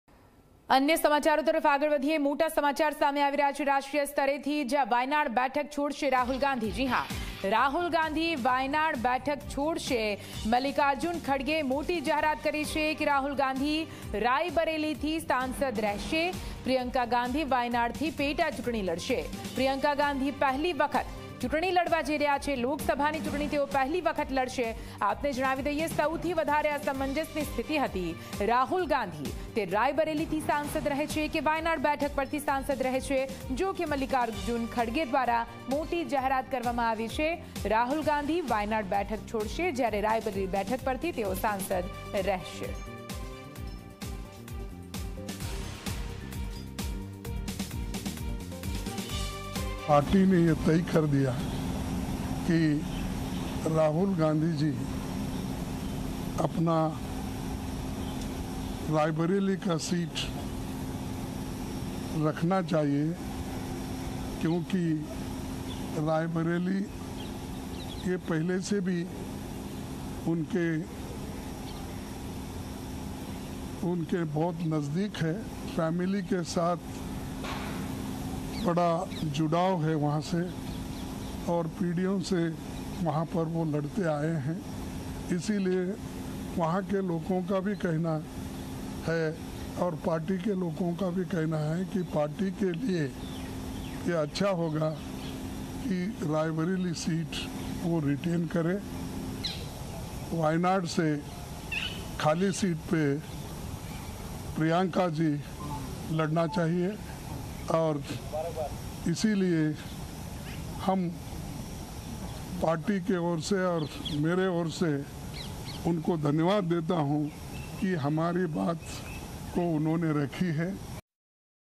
BREAKING: Rahul Gandhi to retain Rae Bareli Lok Sabha seat, Priyanka Gandhi to contest from Wayanad
Rahul Gandhi to retain Rae Bareli Lok Sabha seat, Priyanka Gandhi to contest from Wayanad in bypoll

વાયનાડ બેઠક છોડશે રાહુલ ગાંધી  --

મલ્લિકાર્જન ખડગેની મોટી જાહેરાત  --

રાહુલ ગાંધી રાયબરેલીથી રહેશે સાંસદ  --

પ્રિયંકા ગાંધી વાયનાડથી લડશે પેટાચૂંટણી  ---

પ્રિયંકા ગાંધી પહેલી વખત લડશે ચૂંટણી ---